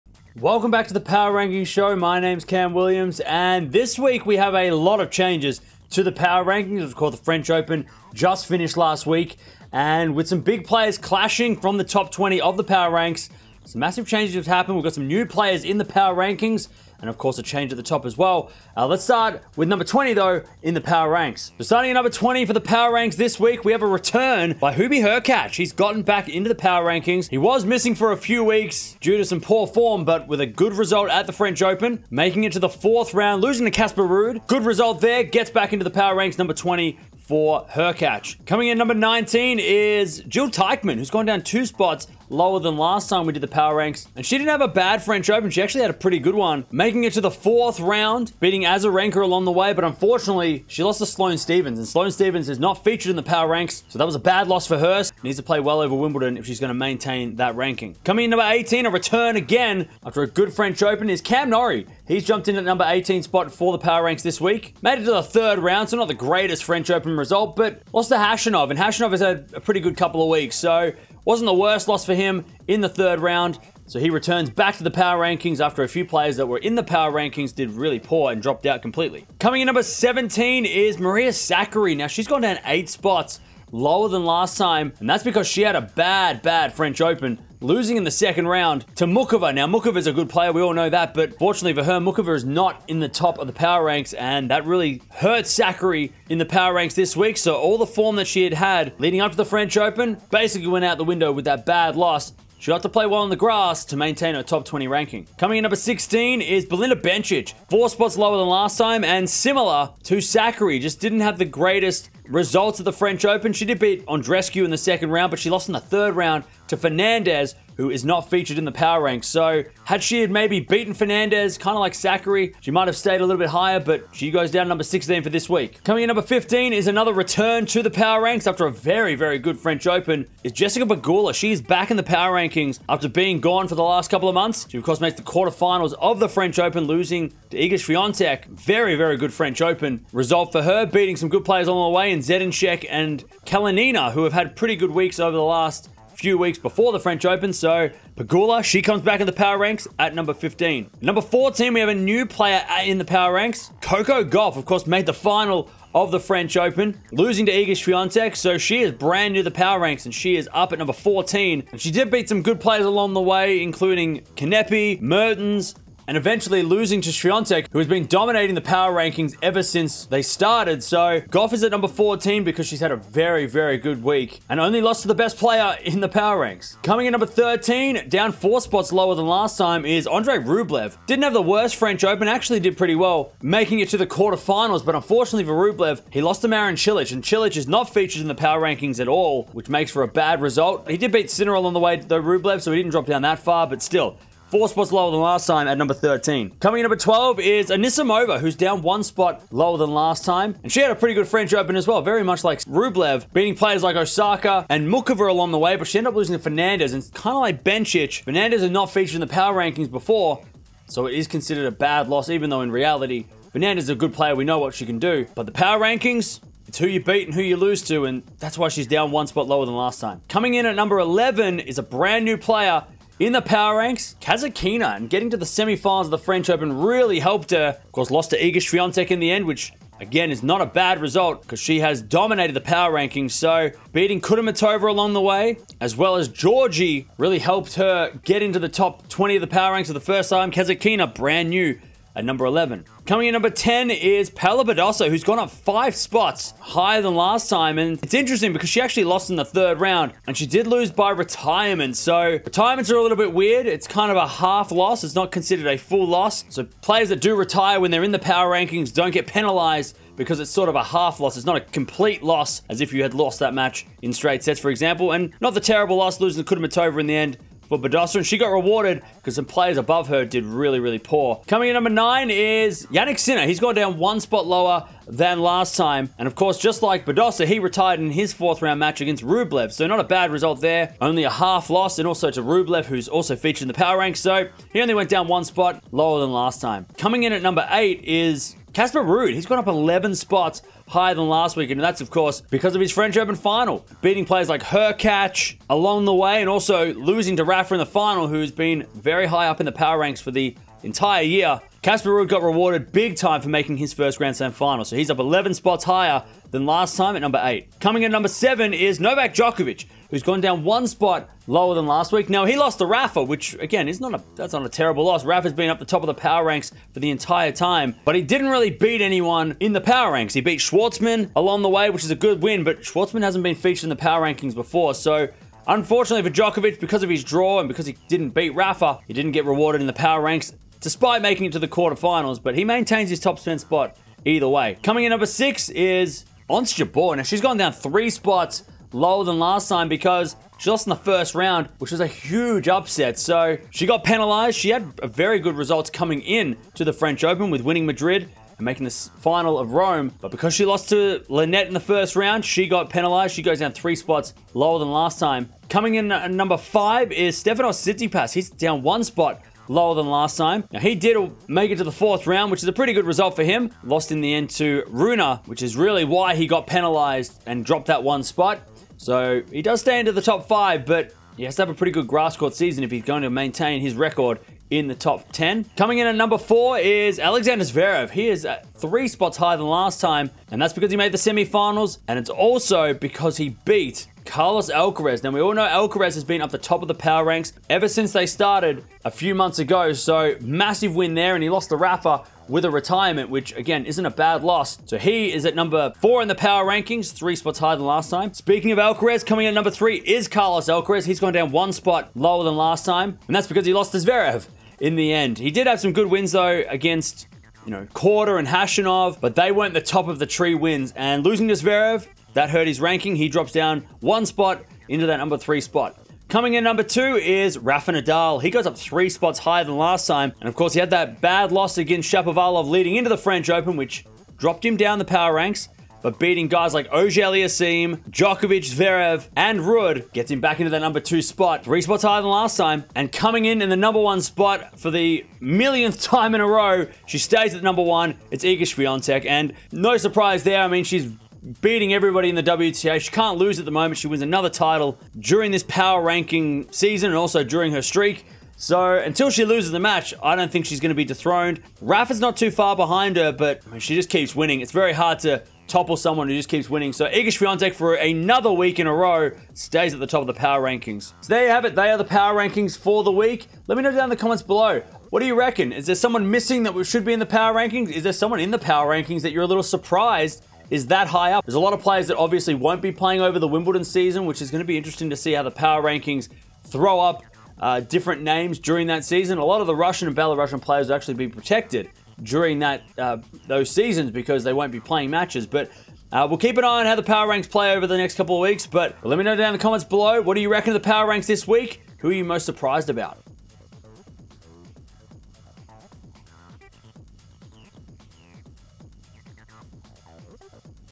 Nadal, Swiatek Dominate | Jabeur Falls after French Open | Tennis Talk Power Rankings
After a crazy two weeks of tennis at Roland Garros 2022 the power rankings have changed dramatically with new faces entering the Top 20 and a change at the top. Who will be number one this week?

0:00 | Intro
0:21 | Number 20
0:38 | Number 19
0:59 | Number 18
1:20 | Number 17
1:46 | Number 16
2:09 | Number 15
2:32 | Number 14
2:59 | Number 13
3:17 | Number 12
3:43 | Number 11
4:04 | Number 10
4:32 | Number 9
4:45 | Number 8
5:07 | Number 7
5:35 | Number 6
5:56 | Number 5
6:16 | Number 4
6:38 | Number 3
6:55 | Number 2
7:13 | Number 1
7:41 | Outro

Power Rankings Explained
1. The Rankings are determined by players wins and loses (Elo Algorithm)
2. If you beat higher ranked player than yourself you will gain more points than if you beat a lower ranked player
3. If you lose to lower ranked player than yourself you will lose more points than if you beat a higher ranked player
4. Walkover and retirements are counted as a loss to that player